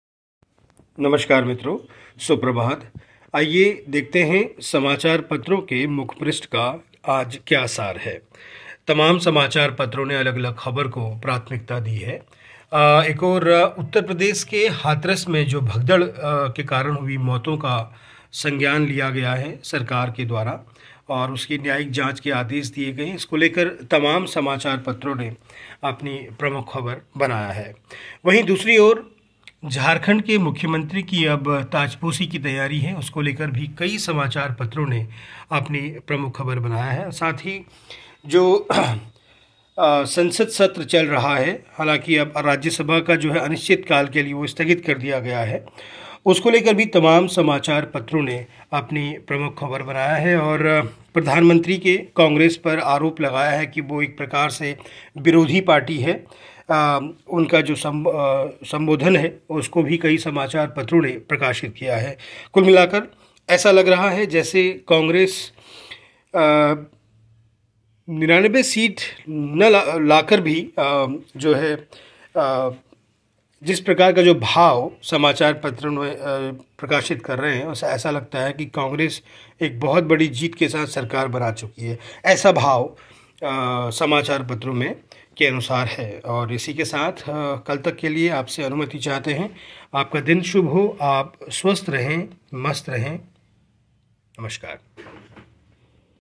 प्रतीत होता है इंडि गठबंधन की सरकार बन गयी है
प्रधानमंत्री नरेंद्र मोदी ने राज्यसभा में बुधवार को राष्ट्रपति के अभिभाषण के धन्यवाद प्रस्ताव पर हुई चर्चा का जवाब दिया। एक घंटे 50 मिनट के भाषण में प्रधानमंत्री ने मणिपुर के हालात, नीट पेपर लीक जैसे मुद्दों पर भी बयान दिया।